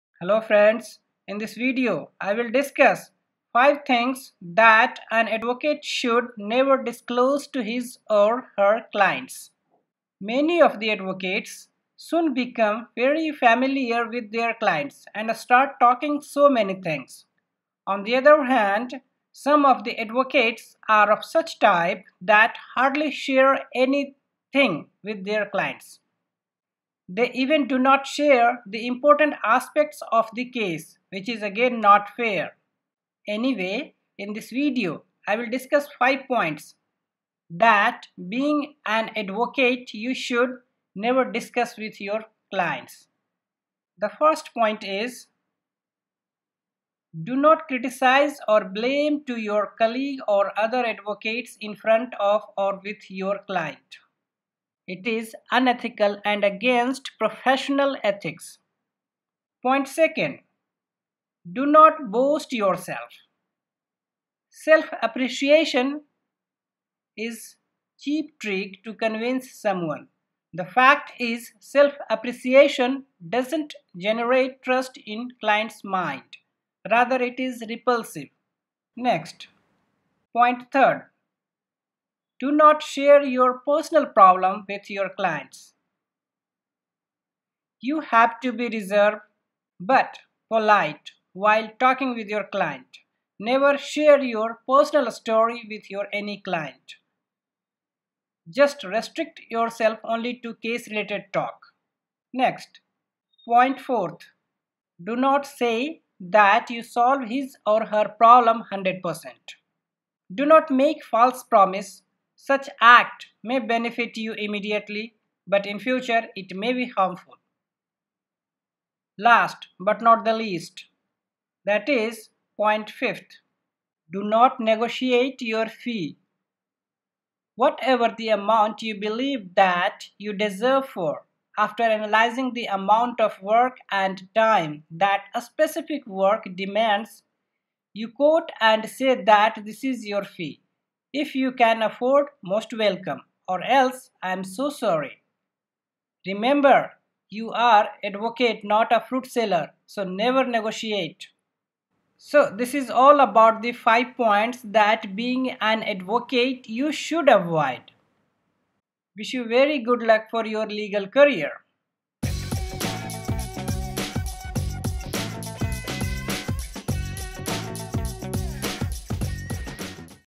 5 Things That An Advocate Should NEVER Disclose to His or Her Clients!
This video will tell you about certain specific acts that being an advocate/lawyer you should avoid.